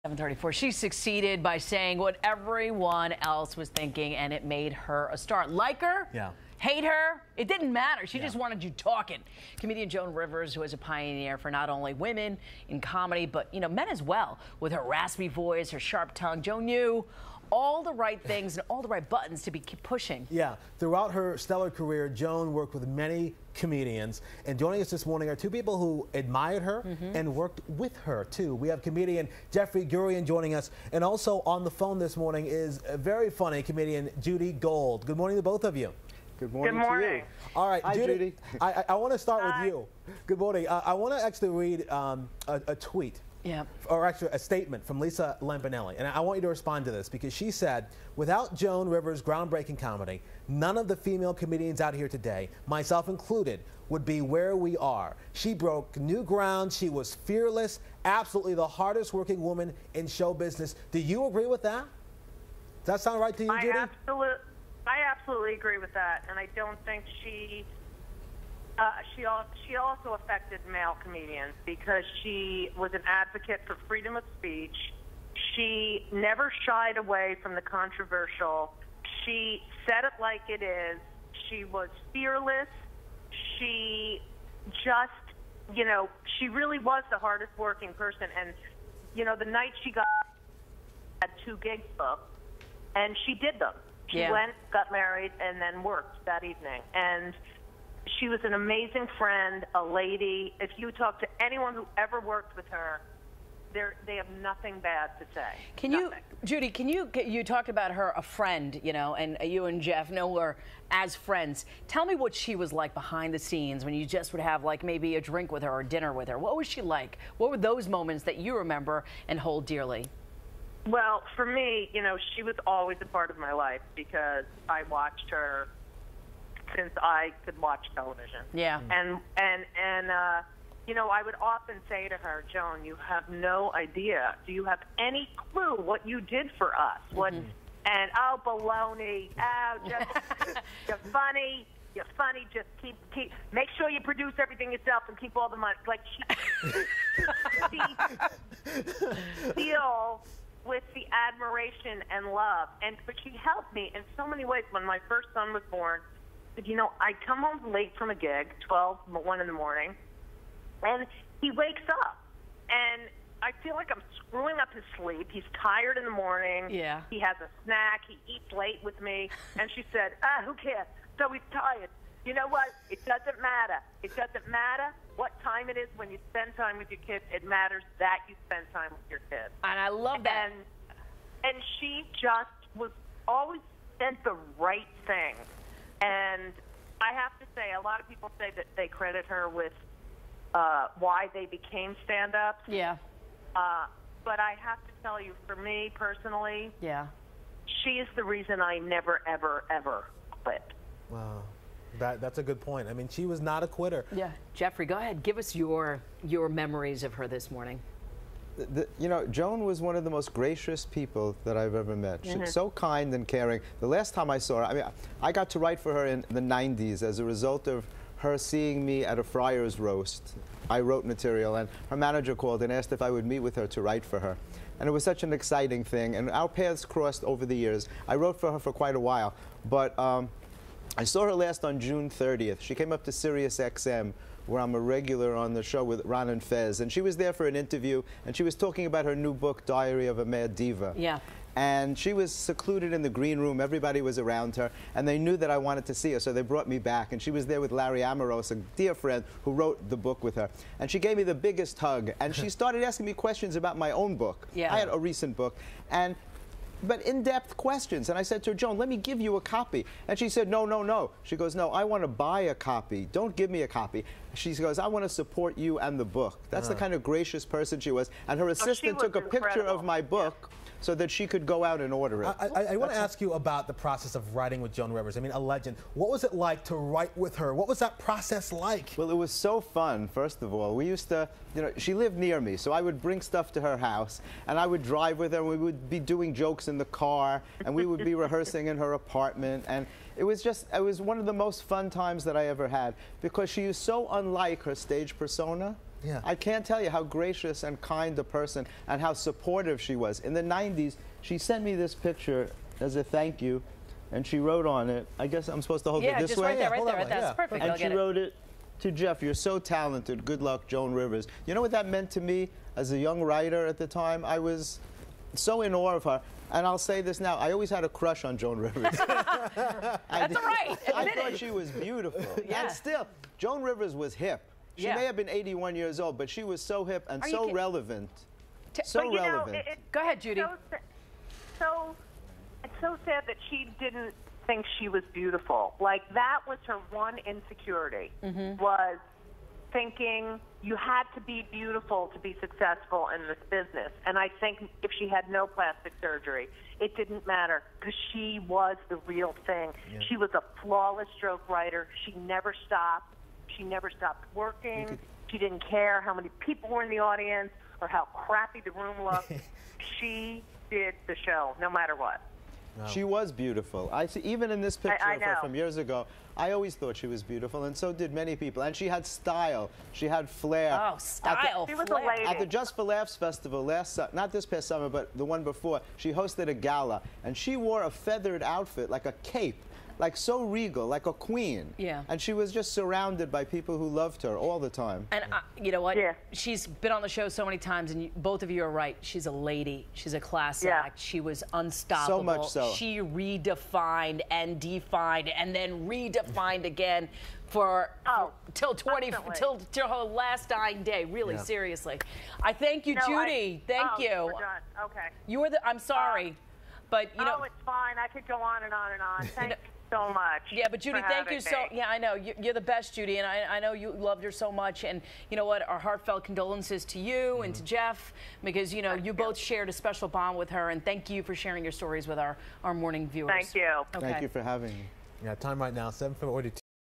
Remembering Joan Rivers w/ Jeffrey Gurian
Jeffrey Gurian, comedy writer/journalist with comedymatterstv.com talking about his memories of Joan Rivers on PIX 11 9/5/14 -Part 1